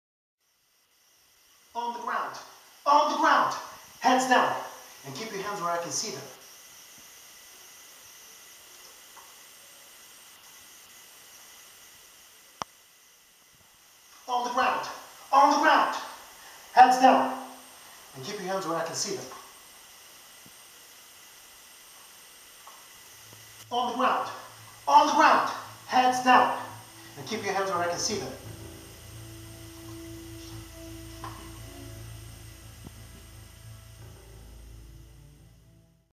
Screen Test: SWEET MADNESS | ACTOR (Slim Khezri) to play SWAT OFFICER in upcoming film!
Slim Khezri shot a quick screen test for a short film, a Harley Quinn (Joker/ Batman​) fan film project, under the working title: "Sweet Madness", produced and directed by Peter Dukes.

Short, quirky, creepy, funny, at times dark, but ultimately entertaining.
The test was for playing a SWAT Team officer.